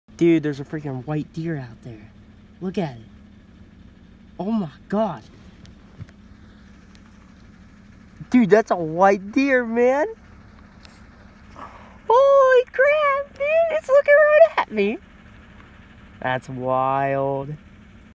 Rare white tailed deer captured today!
My buddy sent me a video of him recording a white white-tailed deer. Amazing!